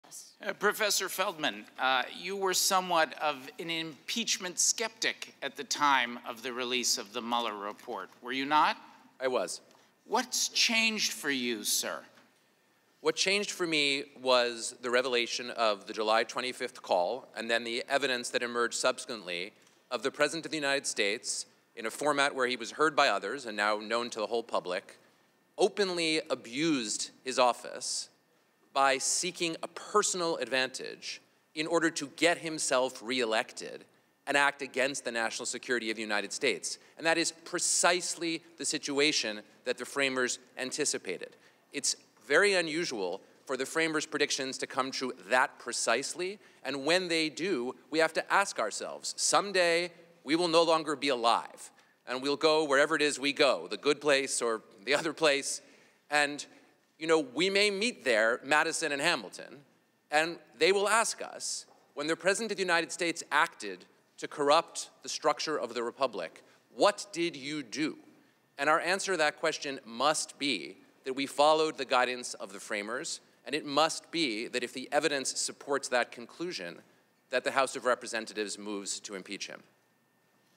WATCH: Trump actions are ‘precisely’ why U.S. founders allowed for impeachment, Feldman testifies
Noah Feldman, a Harvard law professor, told House lawmakers that he was skeptical of impeaching President Donald Trump after the Russia investigation, but changed his views when he learned of the July 25 call, in which Trump asked the president of Ukraine to investigate former vice president and 2020 presidential candidate Joe Biden, and his son, Hunter. Feldman said Trump “openly abused his office by seeking a personal advantage in order to get himself reelected and act against the national security of the United States.” Speaking during a Dec. 4 hearing as part of the impeachment probe into Trump, Feldman said such actions are “precisely” why the nation’s founders allowed for impeachment. The focus of the Judiciary hearing, which came after the House Intelligence Committee had hearings in late November, is to define the grounds for a presidential impeachment.